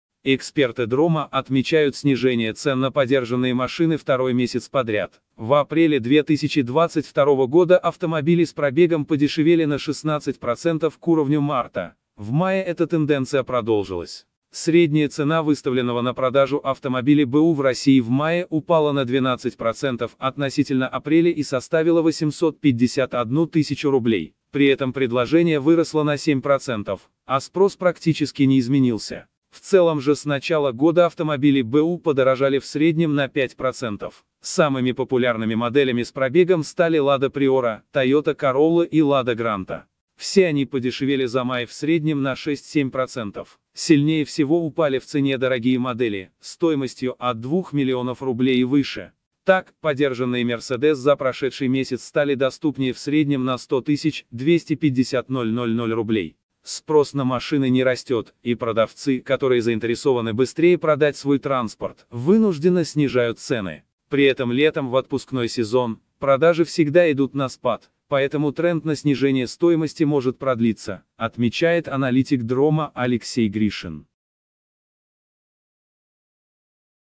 Подержанные машины дешевеют второй месяц подряд
Эксперты Дрома отмечают снижение цен на подержанные машины второй месяц подряд. В апреле 2022 года автомобили с пробегом подешевели на 16% к уровню марта, в мае эта тенденция продолжилась.

Средняя цена выставленного на продажу автомобиля б/у в России в мае упала на 12% относительно апреля и составила 851 000 рублей. При этом предложение выросло на 7%, а спрос практически не изменился. В целом же с начала года автомобили б/у подорожали в среднем на 5%.

Самыми популярными моделями с пробегом стали Lada Priora, Toyota Corolla и Lada Granta. Все они подешевели за май в среднем на 6–7%. Сильнее всего упали в цене дорогие модели, стоимостью от 2 млн рублей и выше. Так, подержанные Mercedes за прошедший месяц стали доступнее в среднем на 100 000–250 000 рублей.

— Спрос на машины не растет, и продавцы, которые заинтересованы быстрее продать свой транспорт, вынужденно снижают цены. При этом летом, в отпускной сезон, продажи всегда идут на спад, поэтому тренд на снижение стоимости может продлиться, — отмечает аналитик Дрома Алексей Гришин.